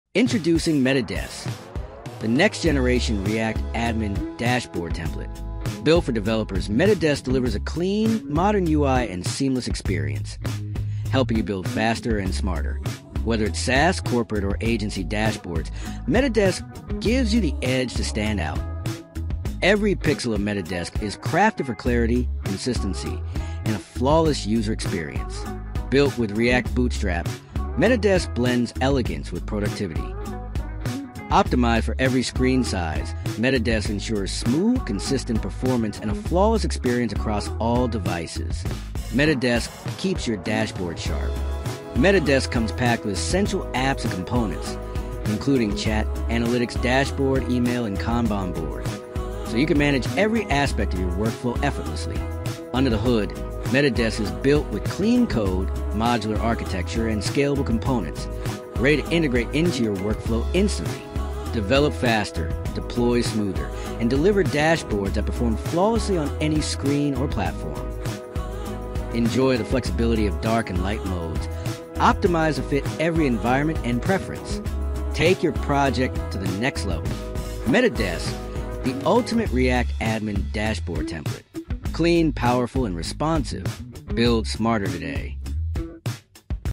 Build Scalable SaaS Apps Fast – Metadesk React Admin Dashboard Template
Metadesk – React Admin Dashboard Template
Metadesk is a modern, clean, and highly customizable React Admin Dashboard Template built with React 19, TypeScript, Vite, and React-Bootstrap (latest). It uses SCSS/SASS for styling and is designed for developers, startups, and businesses who want a scalable, responsive, and professional admin dashboard for their web applications.

Metadesk comes with a fully structured codebase, reusable components, and prebuilt pages that make development faster and easier. It is suitable for building SaaS platforms, CRM/ERP systems, e-commerce backends, analytics dashboards, and more.